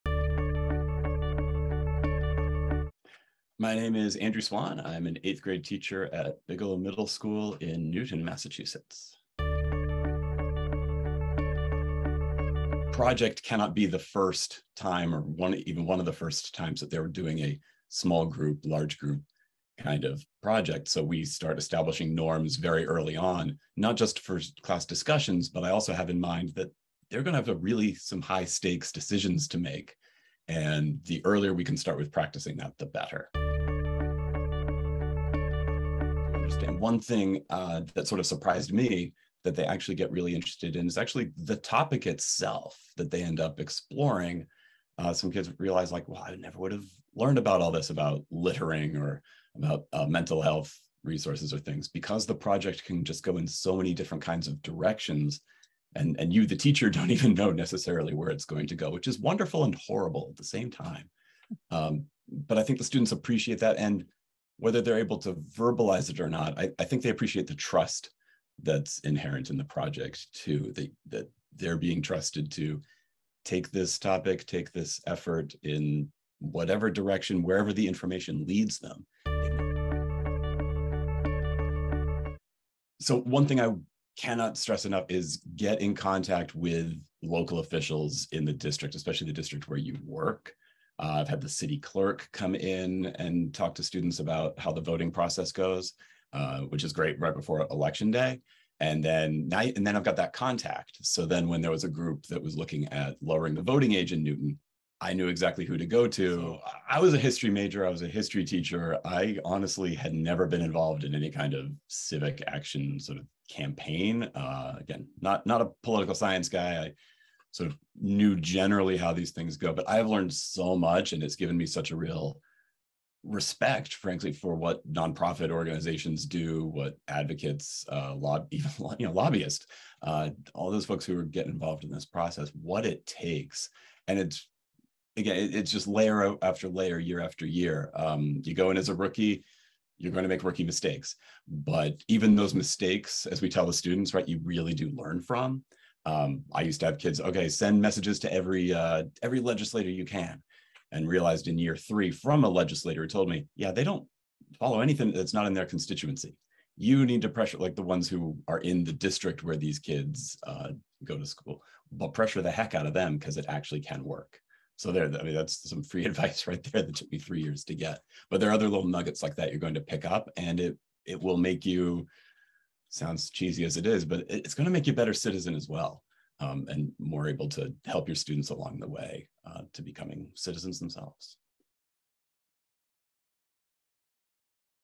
Student-led Civics Project Testimonial: Andrew Swan
Eighth-grade civics teacher and Massachusetts Civics Fellow, Andrew Swan, reflects on and shares his learning about planning and implementing student-led civics projects.